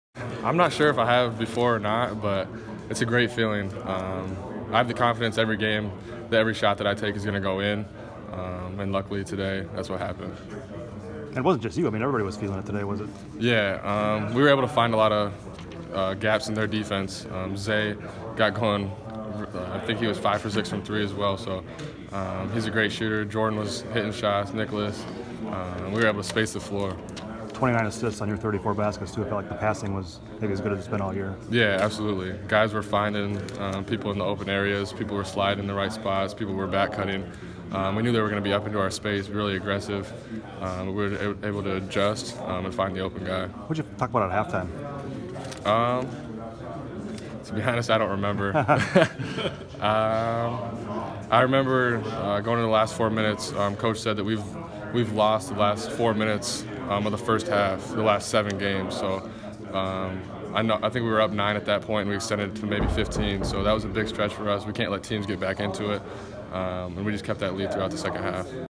Joe Wieskamp on his perfect day of shooting against Illinois
Iowa forward Joe Wieskamp doesn't remember ever having a perfect shooting day. He also says last 4 minutes of the first half were critical.

Read more on HawkCentral.com:
- Wieskamp adds to his legend as No. 24 Iowa sizzles in win over Illinois
- Leistikow: Being good not good enough for Hawkeyes after 5th straight win. 'We want to be great.'